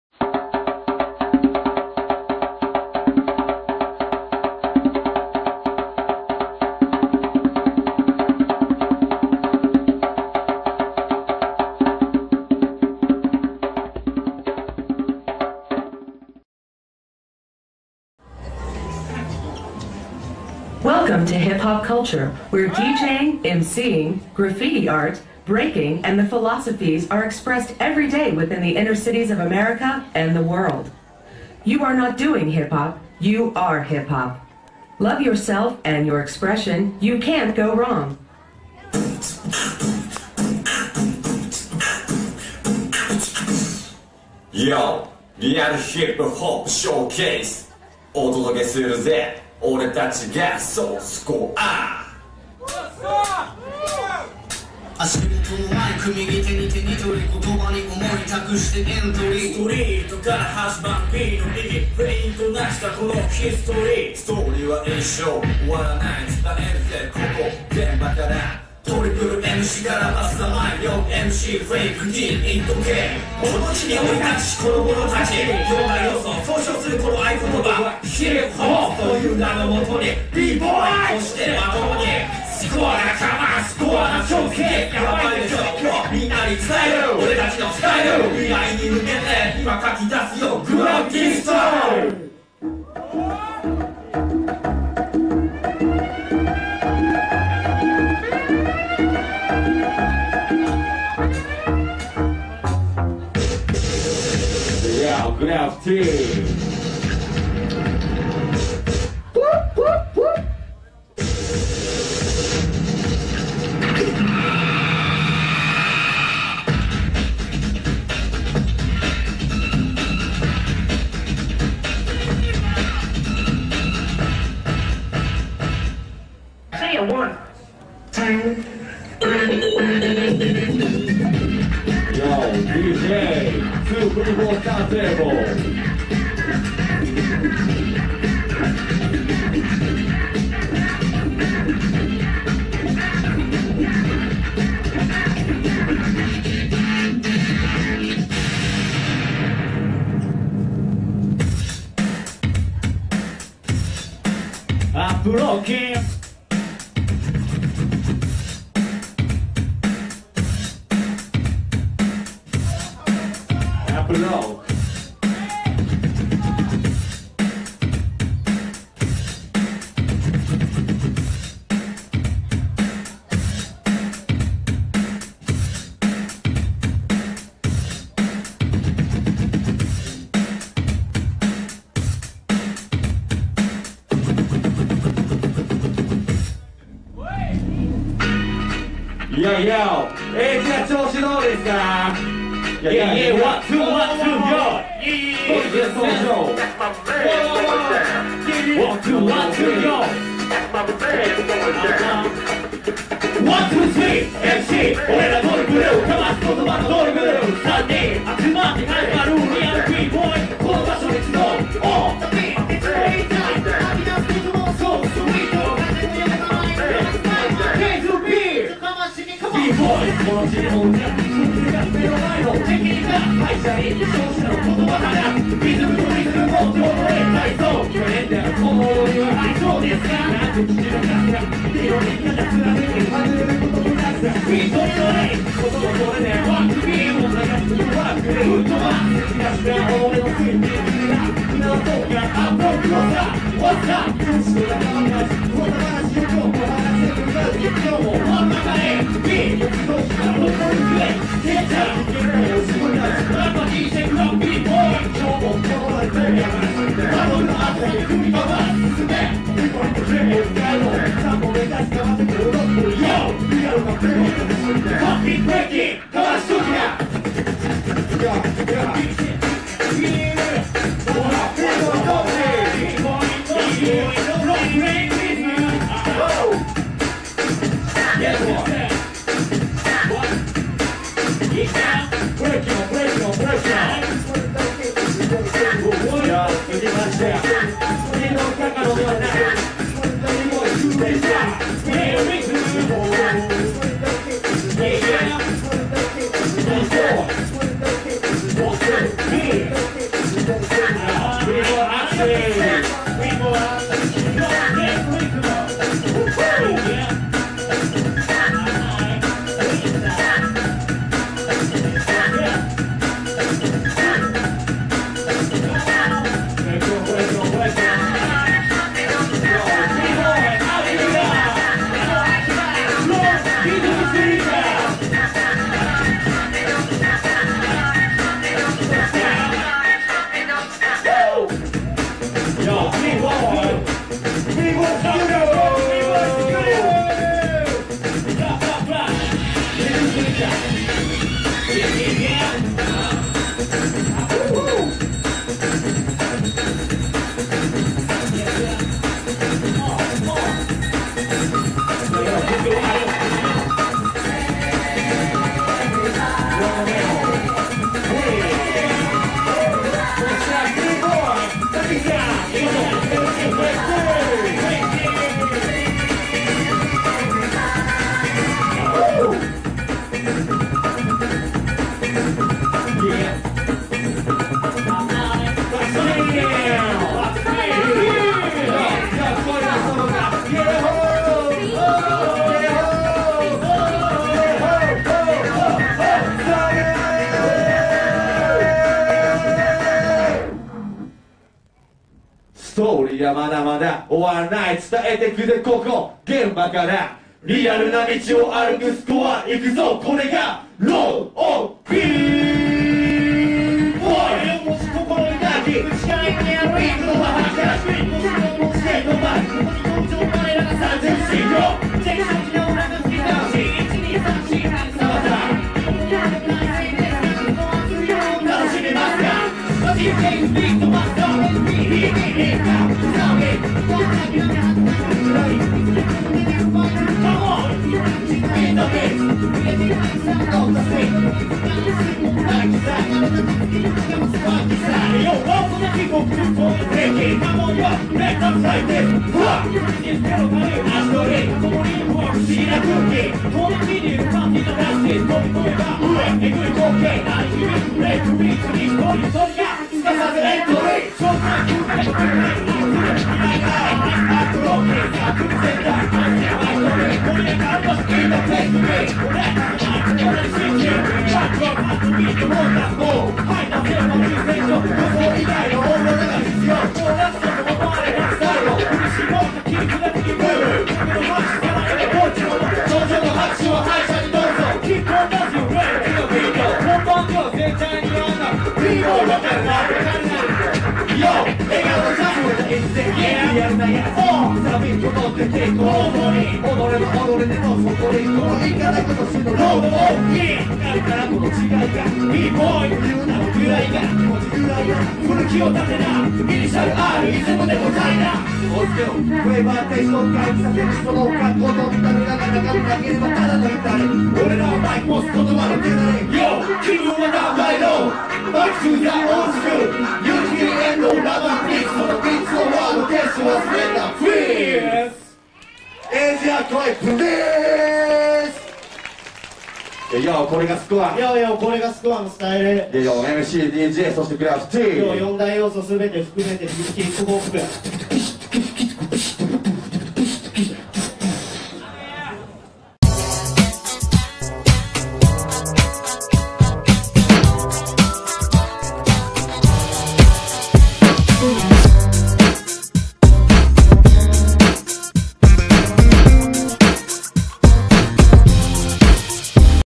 SCORE(TRIPLE/ RHYTHMSNEAKERS/PLAYNRHYTHM) 4 ELEMENTS HIPHOP
TRIPLEとRHYTHMSNEAKERSとPLAYNRHYTHMのユニット🎤👟

SCOREによる4 ELEMENTS HIPHOP SHOW🔥
みんなかなり若い！(笑)

PLAYNRHYTHMは高校生や大学生かな？

STUDIORIZM